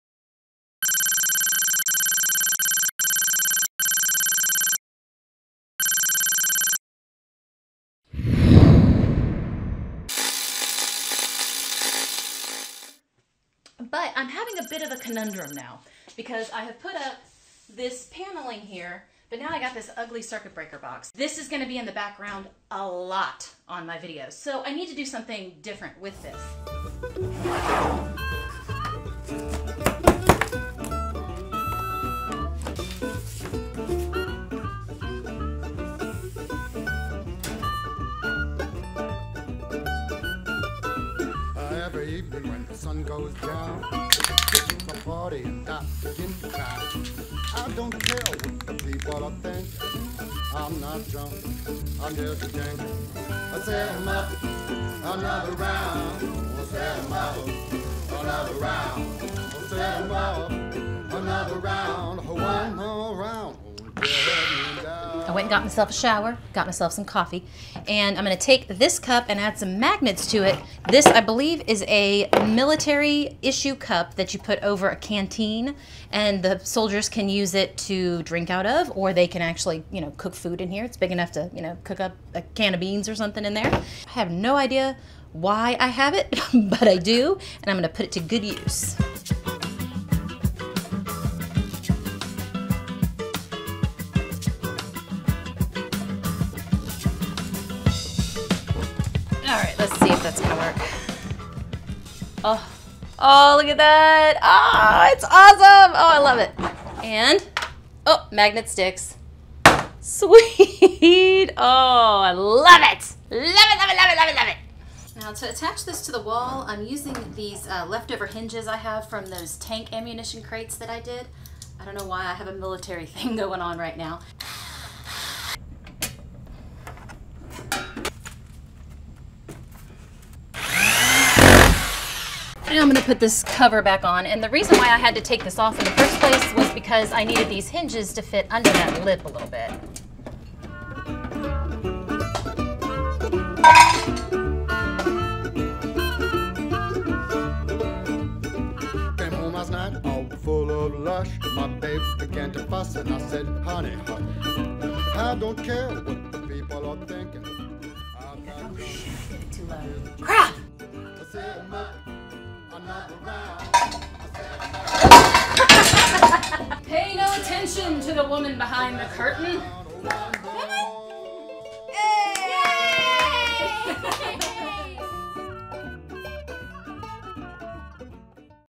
Is it okay to cover your circuit breaker box?
Sometimes your circuit breaker box is in an unfortunate place, and many people cover them with nice decor.  Here is my solution...but does this break electrical code?  Well, kinda.  Watch for more details!